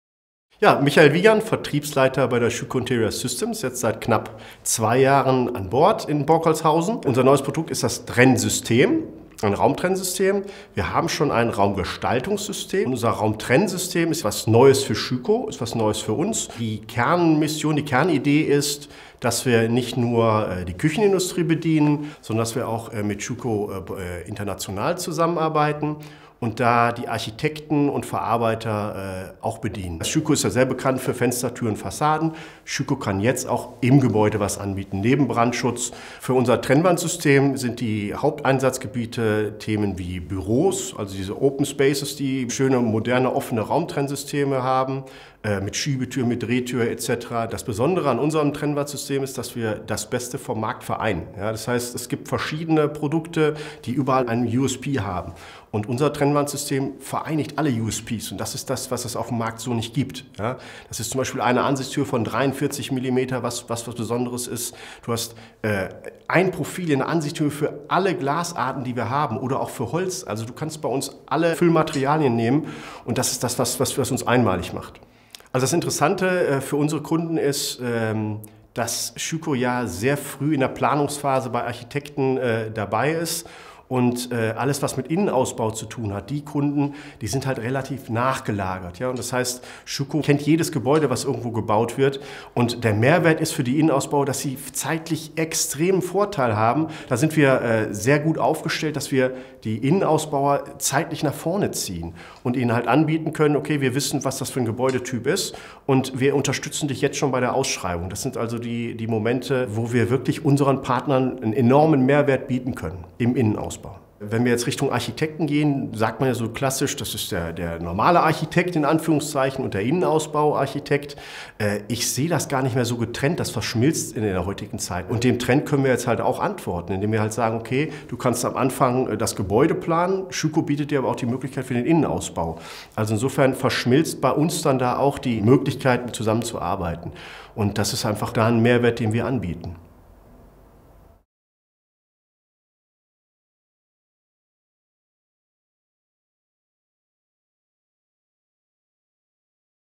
Interview zum neuen Trennwandsystem I Michael Wiegand - Vertriebsleiter Schüco Interior Systems
Das neue Trennwandsystem Schüco Interior AP VS 43 ST ermöglicht Innenraumgestaltung auf höchstem Niveau. Im Interview erzählt Michael Wiegand, Vertriebsleiter Schüco Interior Systems KG, mehr zu den Besonderheiten des Systems und den unterschiedlichen Objektanforderungen, denen es gerecht wird.